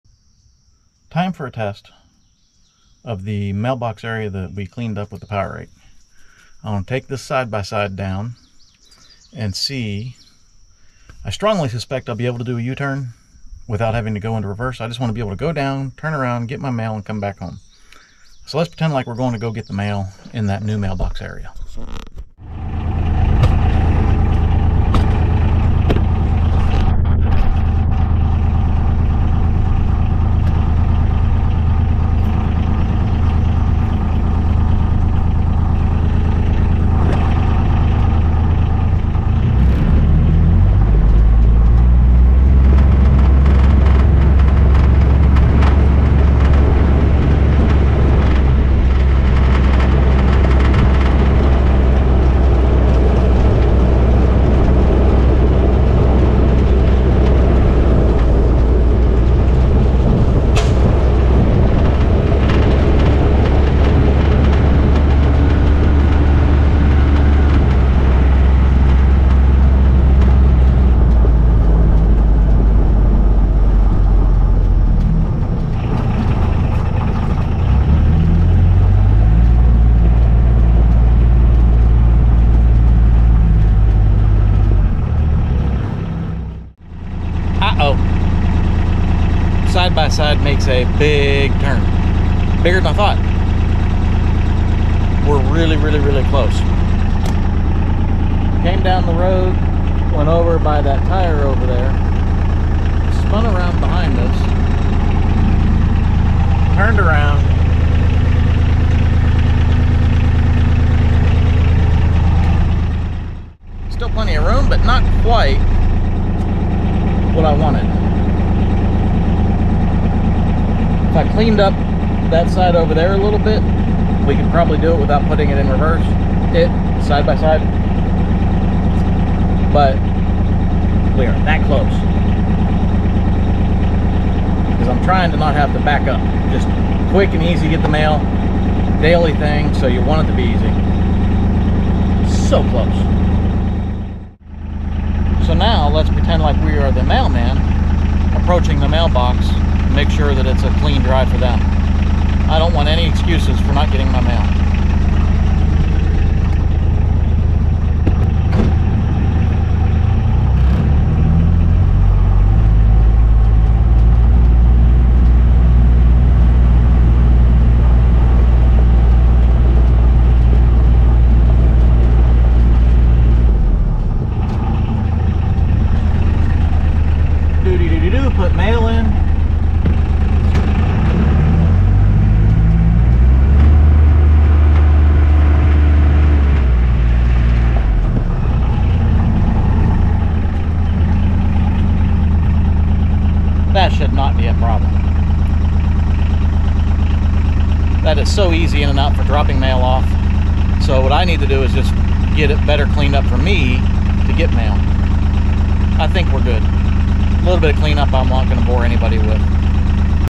New Mailbox Drive Test
Now that the base area is ready for me to put up the mailbox I wanted to make a test drive with the UTV. Idea is getting mail is a daily thing and it needs to be as easy as possible. Daily tasks need to not add frustration to already busy lives.